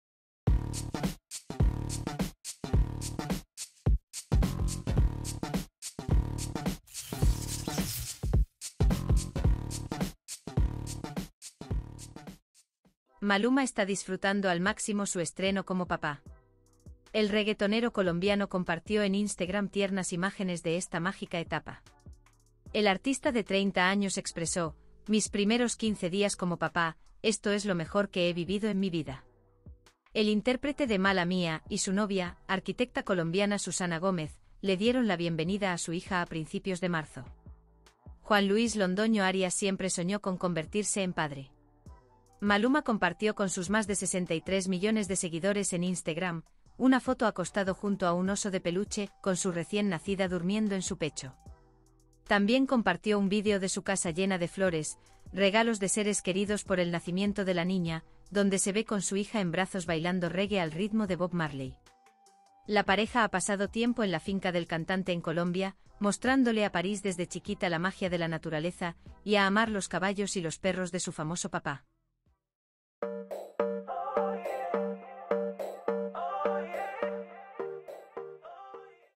Maluma habla de su nueva etapa como papá y muestra la carita de su bebé París
Maluma se estrena como papá y muestra por primera vez el rostro de su hija París ¡Mira la foto!